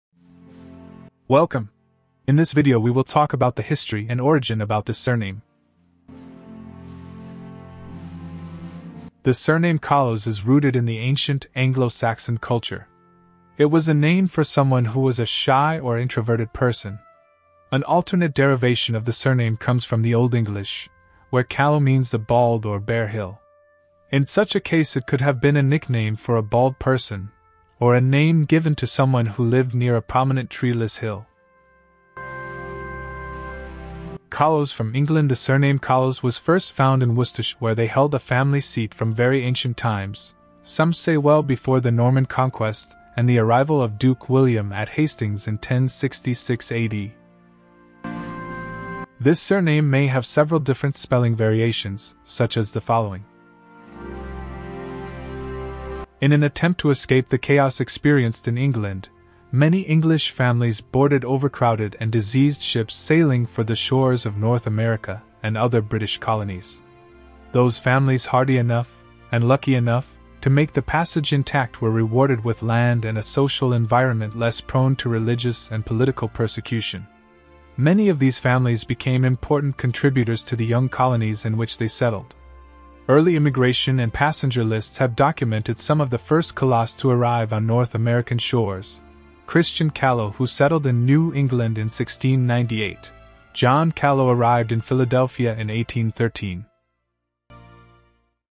Calos from England 🏴󠁧󠁢󠁥󠁮󠁧󠁿 - Meaning, Origin, History & Migration Routes of Surname
The surname Calos is rooted in the ancient Anglo-Saxon culture. It was a name for someone who was a shy or introverted person. An alternate derivation of the surname comes from the Old English, where Callow means the bald or bare hill. In such a case it could have been a nickname for a bald person, or a name given to someone who lived near a prominent treeless hill.

Calos from England The surname Calos was first found in Worcestershire where they held a family seat from very ancient times, some say well before the Norman Conquest and the arrival of Duke William at Hastings in 1066 A.D.